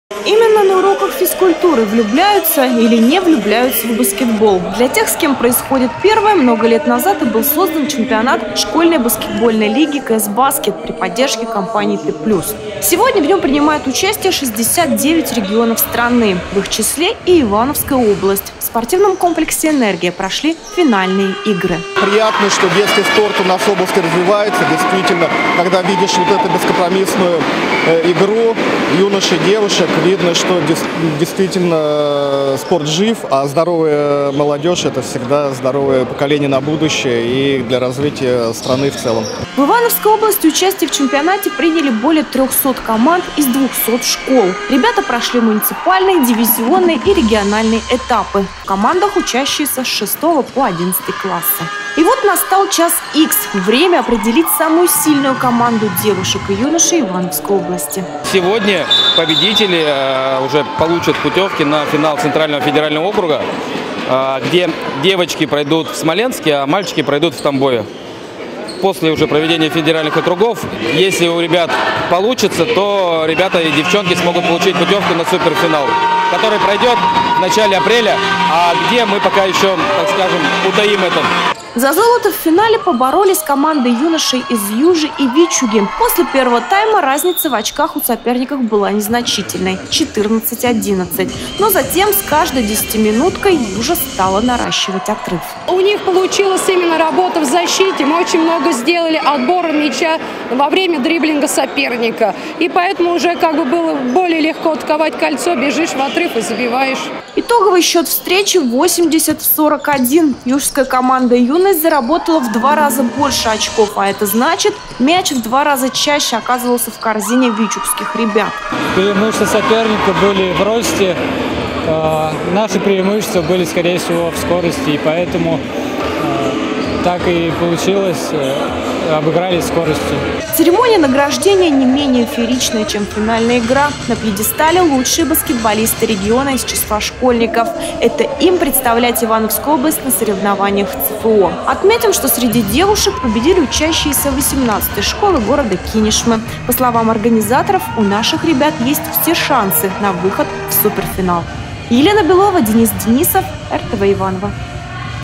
КЭС-БАСКЕТ: Финал
Стали известны победители Чемпионата ШБЛ «КЭС-БАСКЕТ». В финале сразились школьники из Вичуги и Южи. Яркая игра и фееричная церемония награждения –в нашем репортаже.